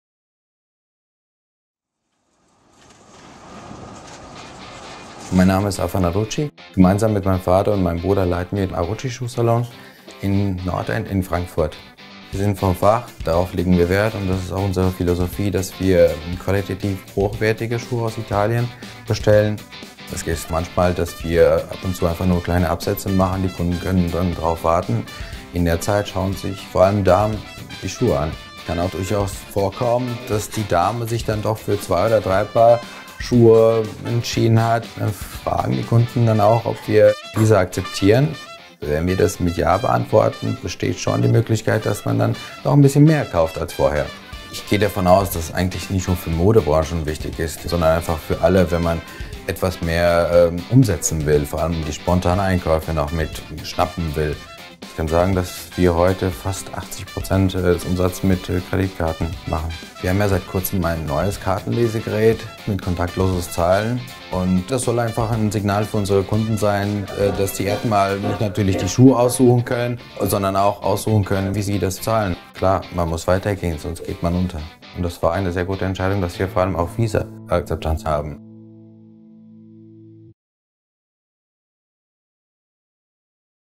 Akzeptanz von Visa Karten kann Kundenumsätze steigern
Der Inhaber des Schuhsalons erklärt, dass sich Kunden - während sie auf die Reparatur eines Schuhs warten -- manchmal zum Kauf neuer Schuhe entscheiden. Die Möglichkeit, mit Visa zahlen zu können erlaubt Kunden ein besseres Managen ihrer Finanzen sowie dem Unternehmen eine Steigerung seiner Gewinne. Und -- das zusätzliche Angebot  der kontaktlosen Zahlungsabwicklung an der Ladenkasse macht den Händler fit für die Zukunft.